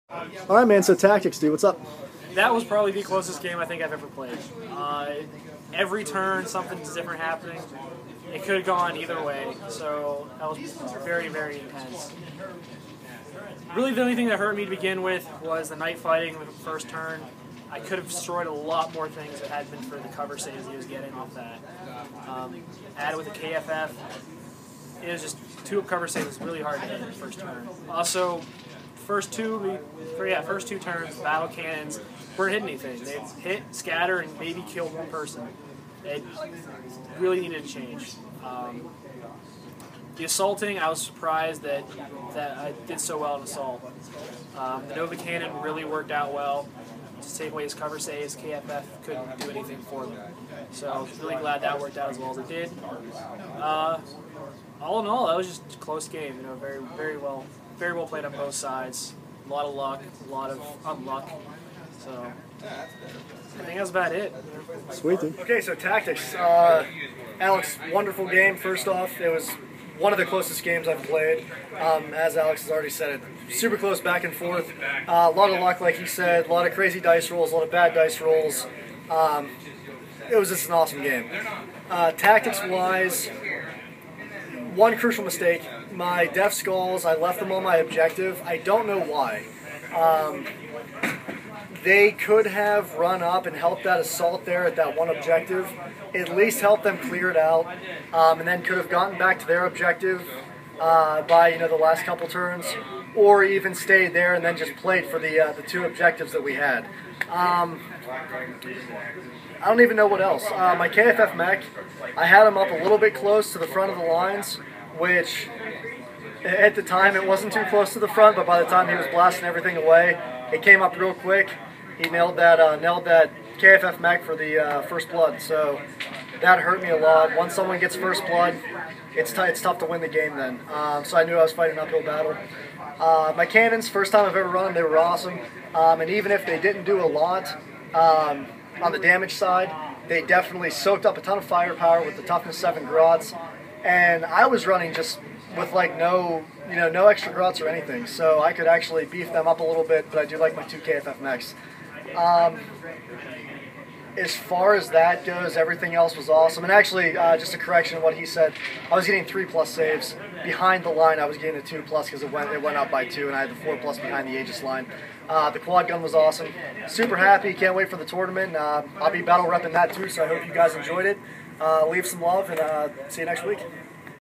Green Tide vs Imperial guard - EPIC Tactics
Green Tide vs Imperial Guard - EPIC end game recap and tactics.  Battle report was too long for the end segment.  Hope you enjoyed the report.  If you didn't watch it, what are you waiting for?